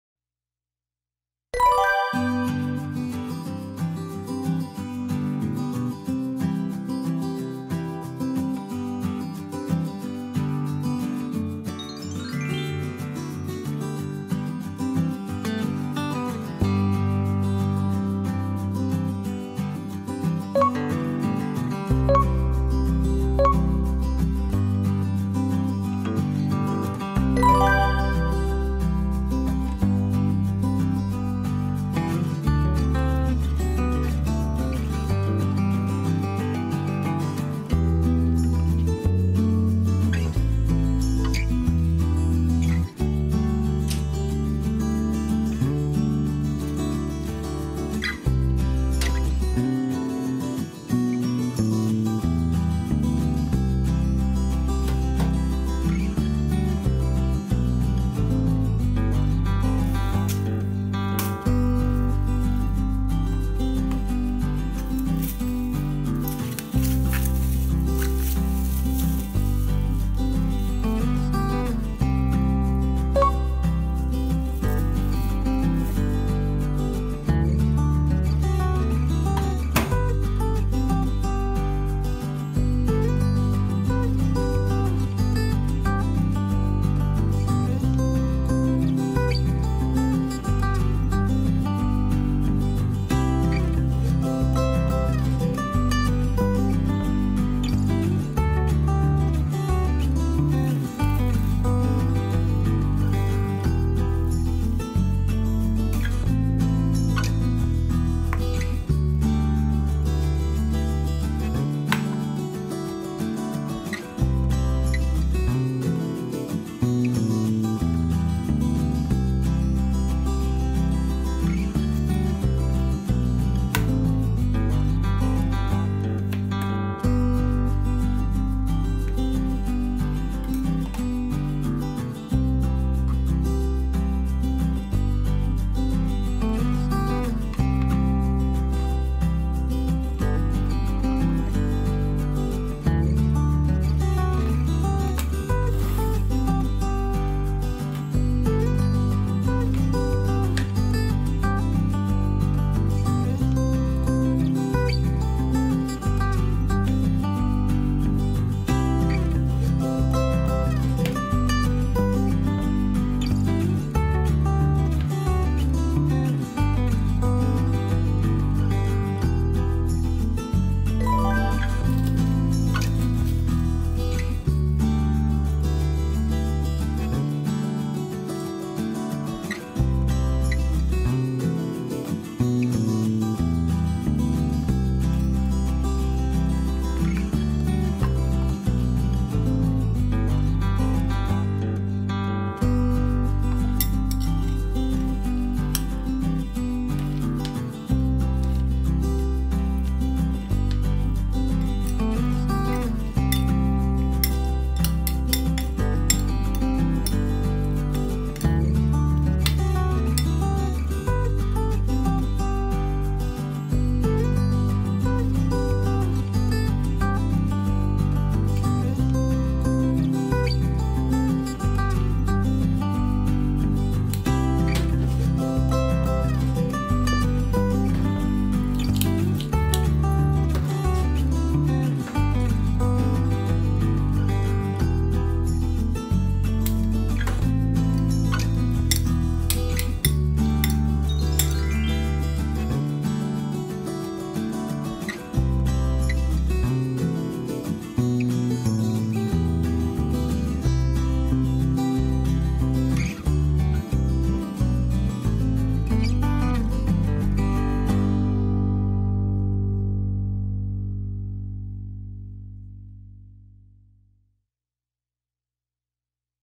レンジでカンタン！美肌になるかぼちゃのサラダ Pumpkin salad
きれいになる成分がたっぷりのかぼちゃのサラダです。電子レンジでカンタンに作れます。

Pumpkin salad with plenty of beautiful ingredients. You can easily make it in the microwave.